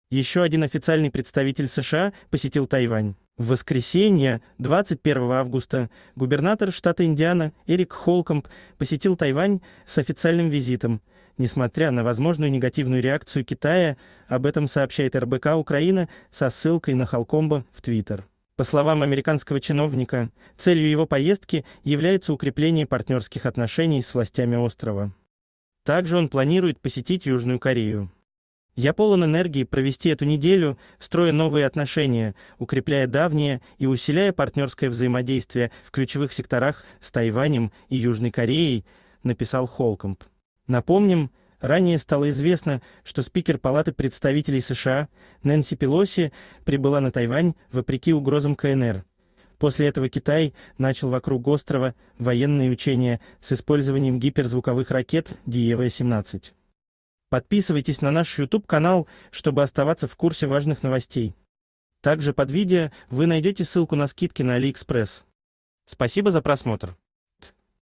Еще один официальный представитель США посетил Тайвань.
Китай Тайвань СоединенныеШтатыАмерики Получи скидку на Алиэкспресс по этой ссылке до 70% + дополнительные скидочные купоны от продавцов. В воскресенье, 21 августа, губернатор штата Индиана Эрик Холкомб посетил Тайвань с официальным визитом, несмотря на возможную негативную реакцию Китая. Об этом сообщает РБК-Украина со ссылкой на Холкомба в Twitter. По словам американского чиновника, целью его поездки является укрепление партнерских отношений с властями острова. Также он планирует посетить Южную Корею. "Я полон энергии провести эту неделю, строя новые отношения, укрепляя давние и усиляя партнерское взаимодействие в ключевых секторах с Тайванем и Южной Кореей", - написал Холкомб. Напомним, ранее стало известно, что спикер Палаты представителей США Нэнси Пелоси прибыла на Тайвань вопреки угрозам КНР. После этого Китай начал